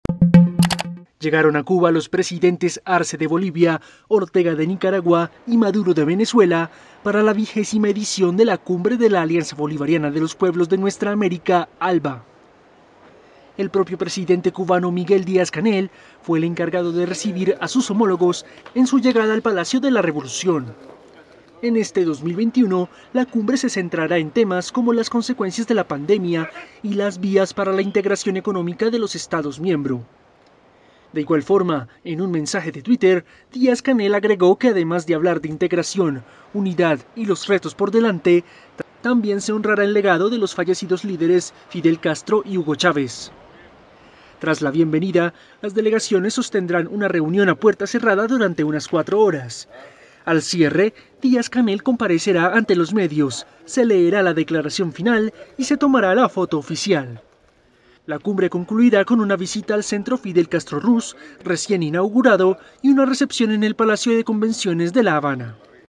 Llegada de Ortega con su hijo y otros presidentes a Cuba en la XX cumbre del ALBA
El presidente de Cuba, Miguel Díaz-Canel, recibió este martes a sus homólogos de Venezuela, Nicaragua y Bolivia, Nicolás Maduro, Daniel Ortega y Luis Arce, a su llegada al Palacio de la Revolución para la XX cumbre de la Alianza Bolivariana de los Pueblos de Nuevas América (ALBA).